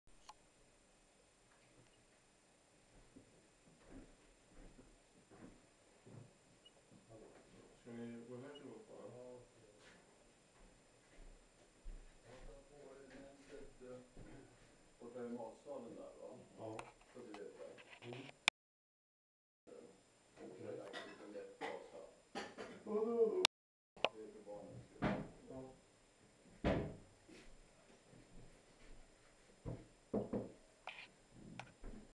Ekorrar leker på eken
Två söta ekorrar som springer runt på eken.